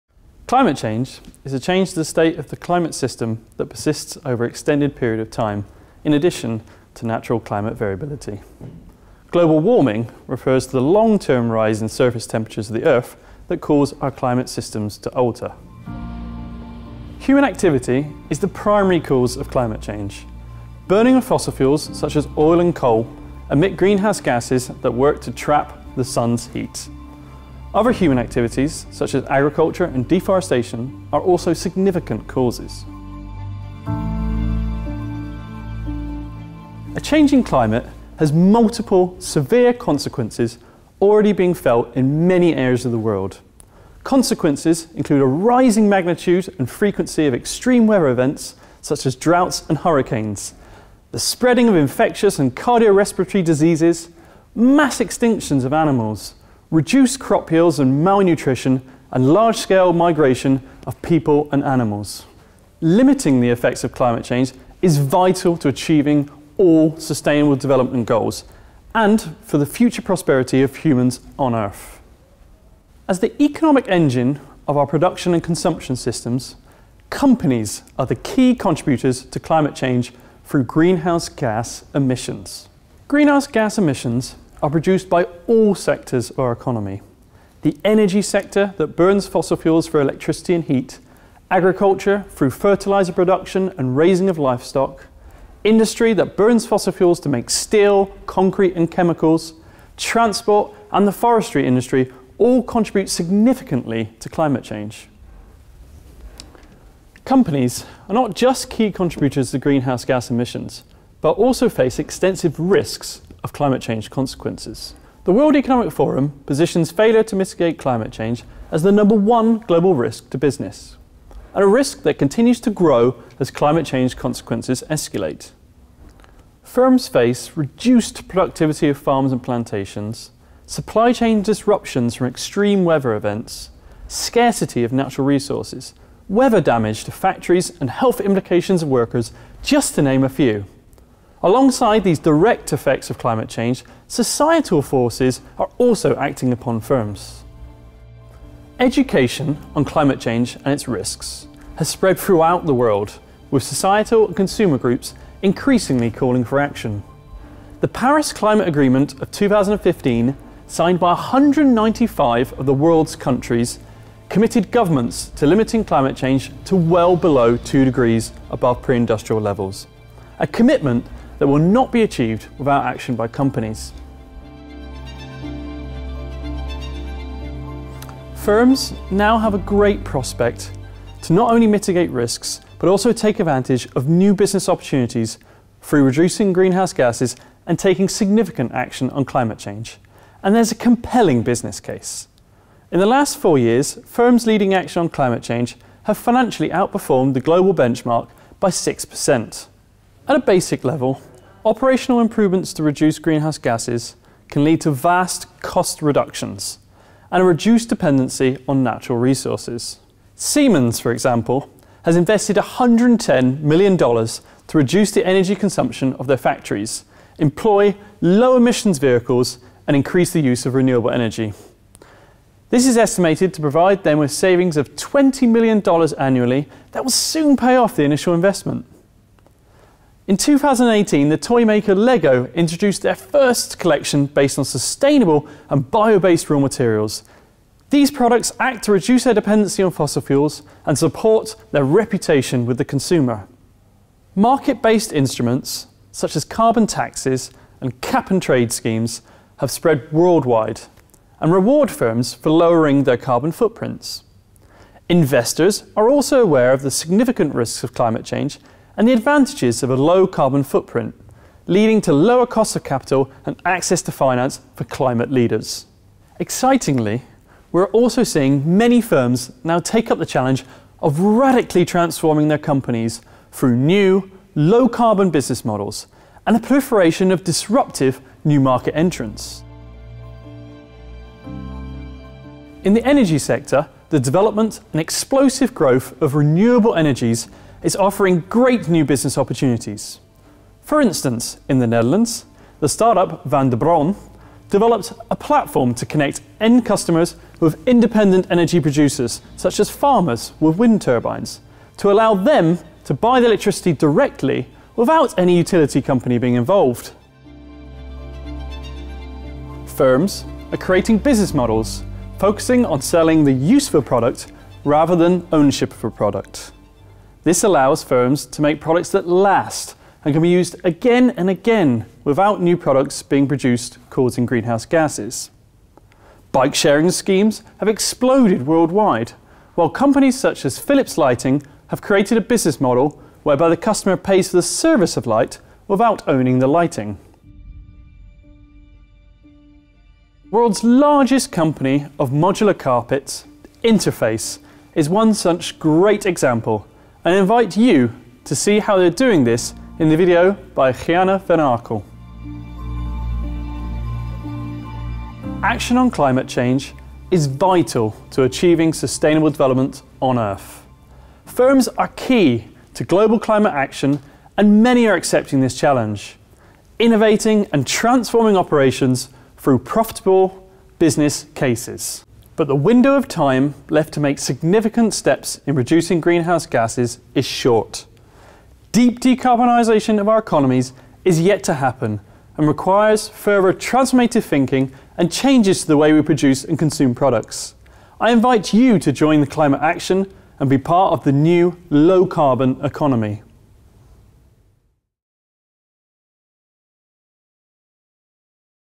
SDG 13: Academic insights into climate action
Rotterdam School of Management, Erasmus University (RSM) developed a series of online learning modules on the UN's Sustainable Development Goals. This is the academic video on SDG 13 Climate Action.